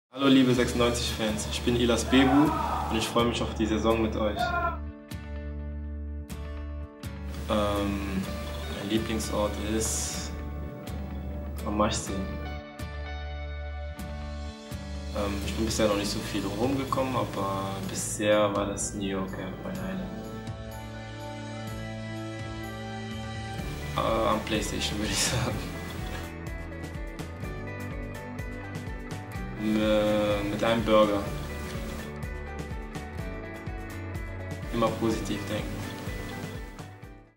Spieler-Porträt 2018/19 | Ihlas Bebou
Was ist Dein Lieblings-Reiseziel? Was ist Dein Lebensmotto? Lernt hier unsere H96-Trainer und -Profis besser kennen. Viel Spaß dabei!